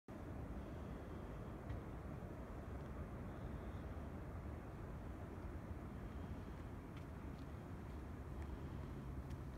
Grayland Beach State Park, WA from Colleen T.
A look into a loop and back to bathrooms on main road of camp

▶︎ Beautiful Oceanfront Campground!
Grayland Beach State Park is made up of an older section, farther from the beach, and a newer section, up against the dunes. As campers leave, volunteers come through to pick up and rake each site. Sites are private and many have woodsy borders. Easy beach trails.